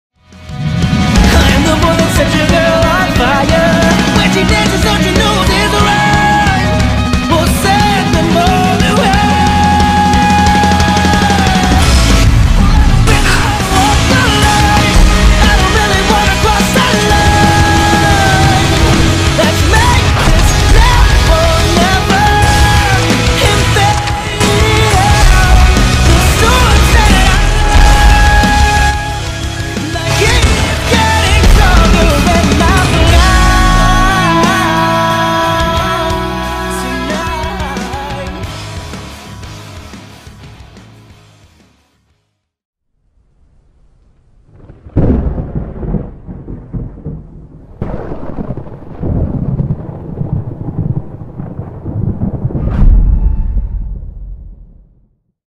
Mixclip III
Uploaded on November 22th, 2013.
Description was as it follows: "Sup, final clip for Dopey's comp, I think it's enjoyable. Had to render twice because audio was fucking up, and Haze intro is in the end because I forgot it."